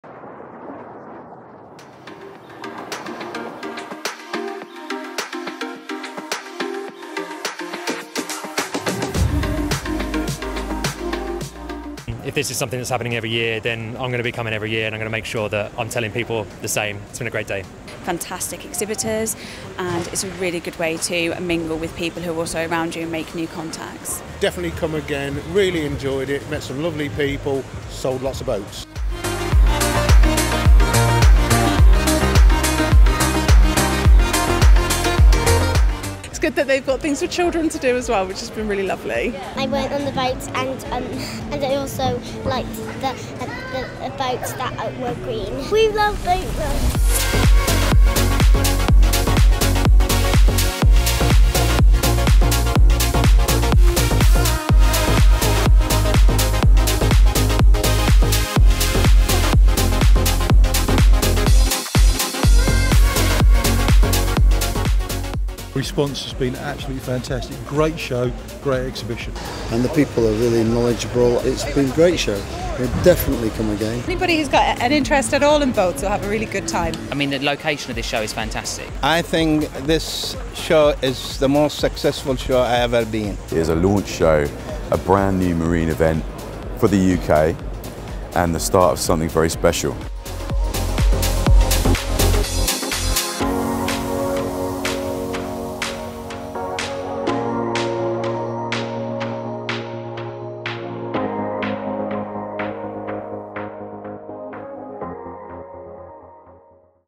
BoatLife Live 2022
BoatLife Live, the brand new event took place in Birmingham from 17 - 20 February 2022. Up to 10,000 people visited the four day event at the NEC listening to daily talks on the Live Stage, trying watersports at the Virtual Reality feature and experiencing boats of every shape and size to suit all boaters.
This is what the show looked like and what our show visitors and exhibitors thought.